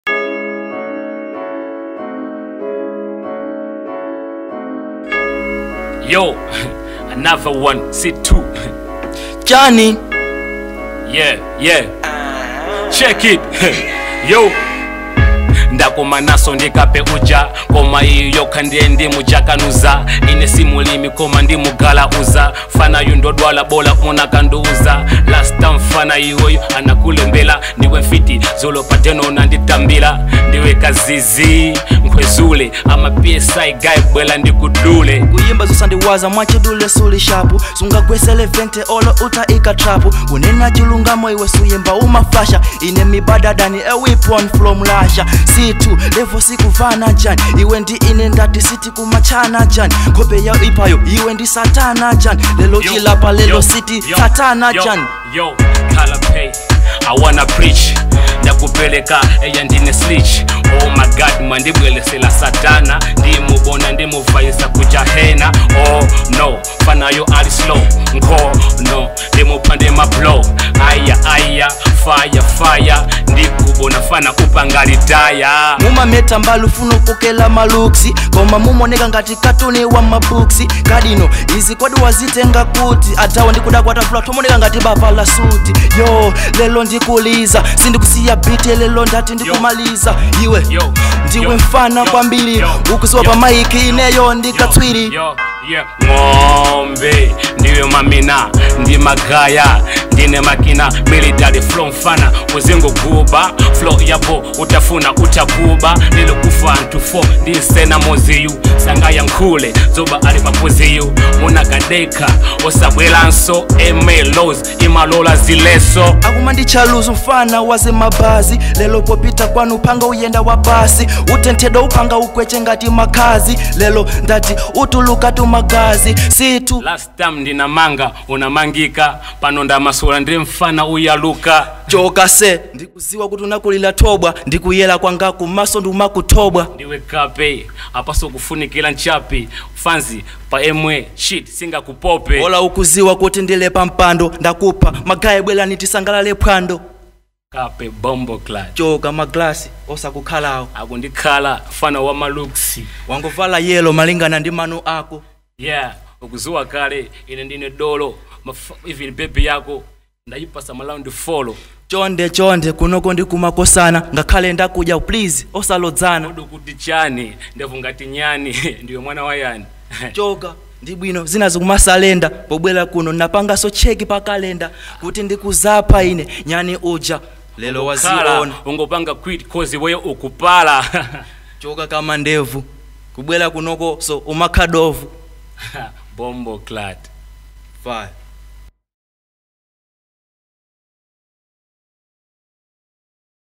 C Too VS Leventy REMATCH On Makosana Mic Battle Level llI
Makosana Mic Battle: it's a One on One Mic Battle to see Who is Bad.

our Facebook pages: Makosana